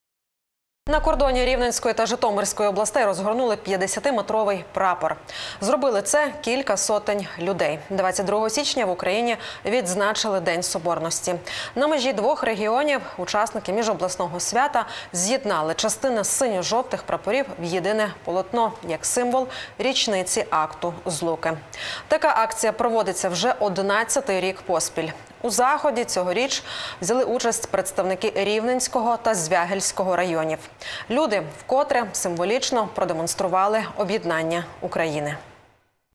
На кордоні Рівненської та Житомирської областей, розгорнули 50- ти метровий прапор
Зробили це кілька сотень людей. 22 січня в Україні відзначили День Соборності. На межі двох регіонів учасники міжобласного свята з’єднали частини синьо-жовтих прапорів в єдине полотно – як символ річниці Акту Злуки. Така акція проводиться вже одинадцятий рік поспіль. У заході цьогоріч взяли участь представники Рівненського та Звягельського районів. Люди вкотре символічно продемонстрували об’єднання України.